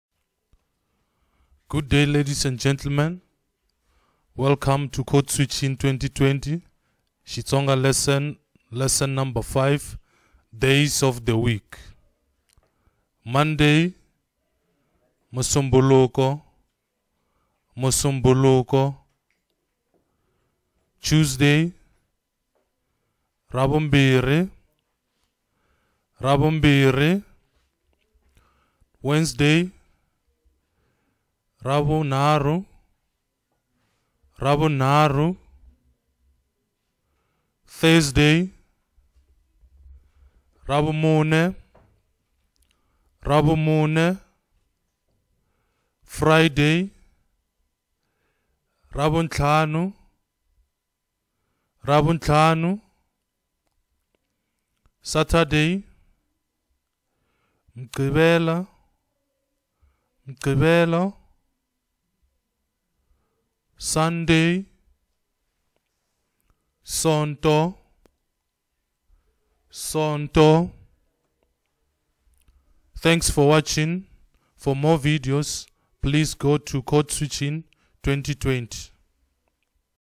XITSONGA LESSON NO 5 DAYS OF THE WEEK
This video Codeswitch Xitsonga days of the week, for more videos go to Codeswitching2020.